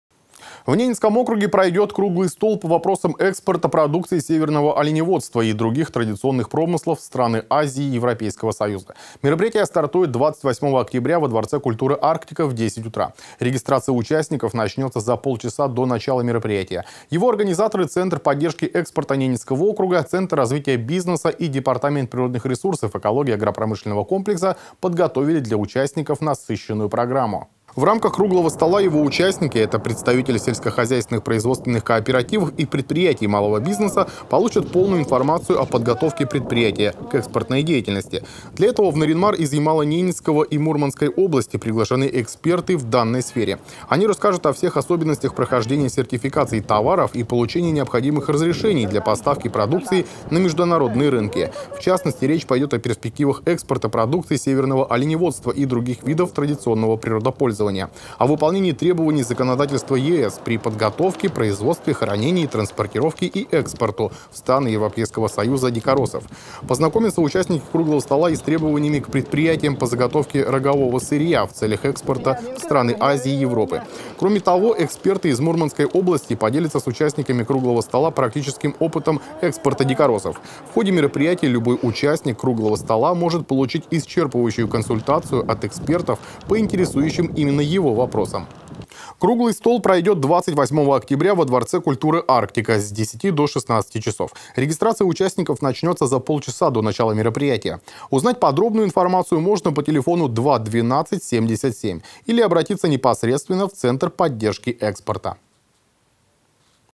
ПУТЬ НА ЭКСПОРТ
В Ненецком округе пройдет круглый стол по вопросам экспорта продукции северного оленеводства и других традиционных промыслов в страны Азии и Европейского Союза. Мероприятие стартует 28 октября во Дворце культуры "Арктика" в 10 утра(регистрация участников начинается за полчаса до начала мероприятия). Его организаторы Центр поддержки экспорта Ненецкого округа, Центр развития бизнеса и Департамент природных ресурсов, экологии и агропромышленного комплекса подготовили для участников насыщенную программу.